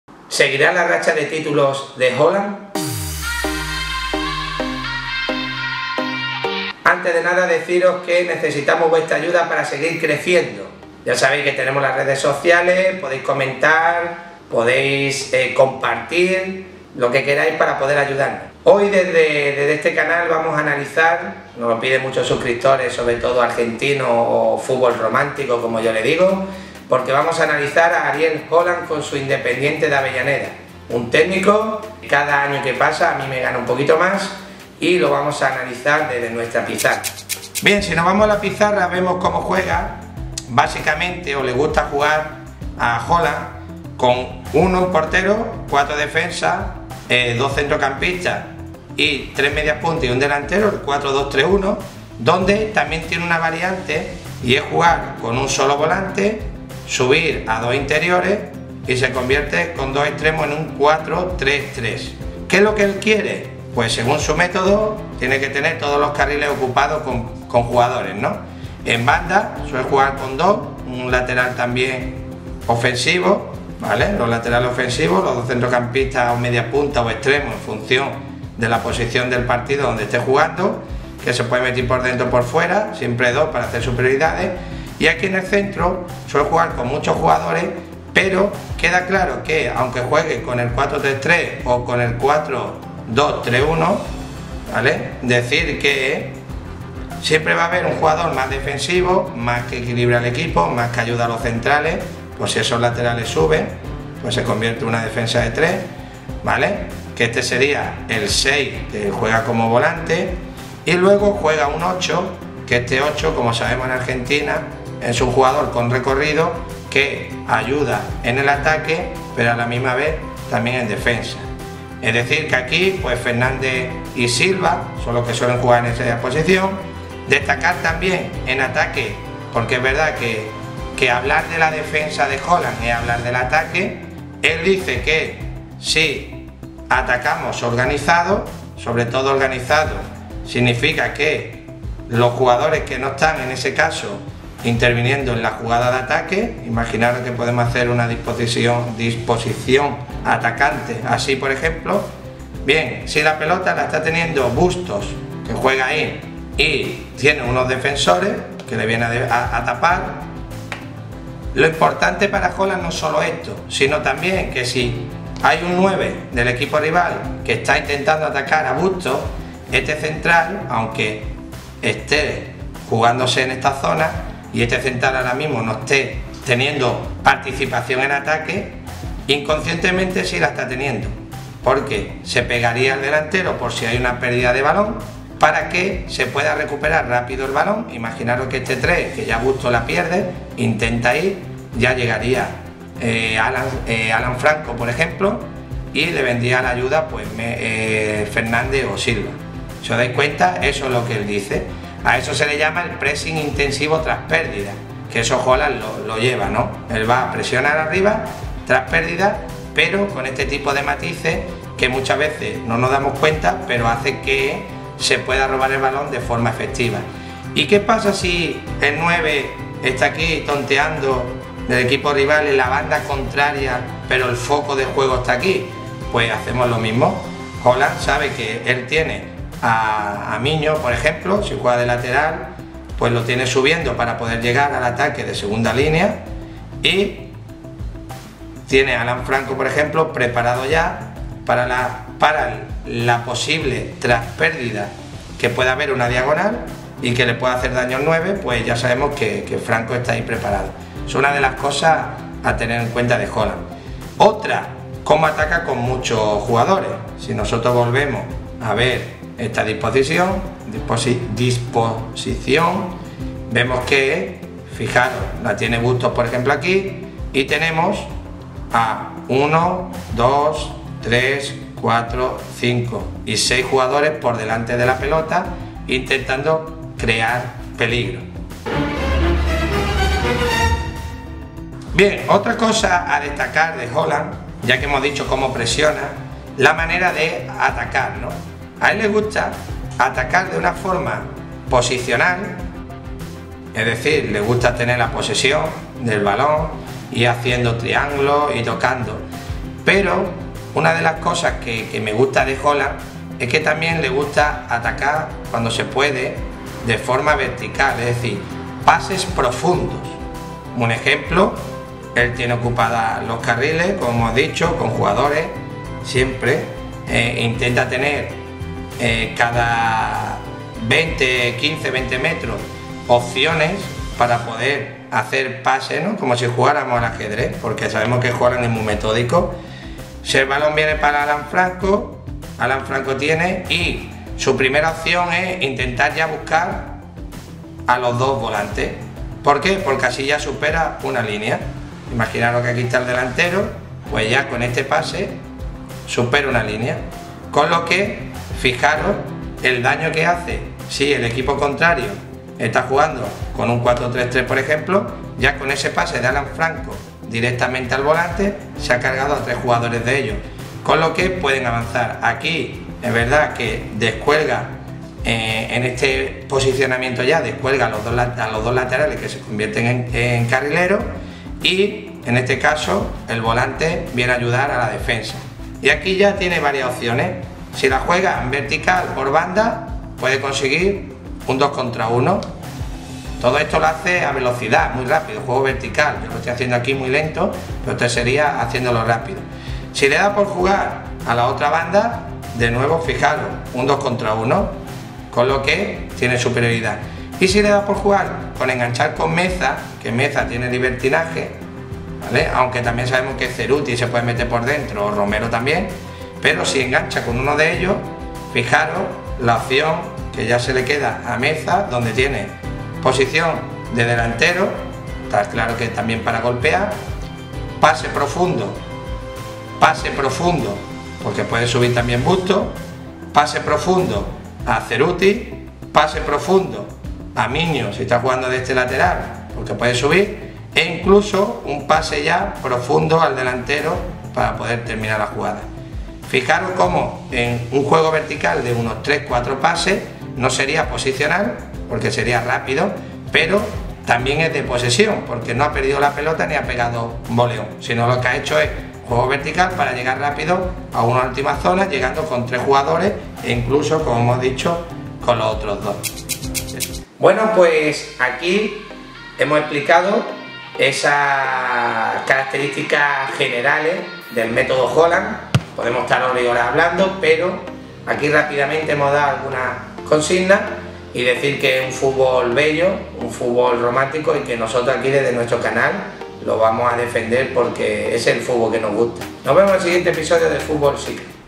Ariel Holan | Análisis Táctico ( Independiente de Avellaneda )
Ariel Holan ha guiado en los últimos años a Independiente de Avellaneda a lugares insospechados. Hemos estudiado durante mucho tiempo su táctica y sistema de juego que hemos plasmado en nuestra pizarra táctica.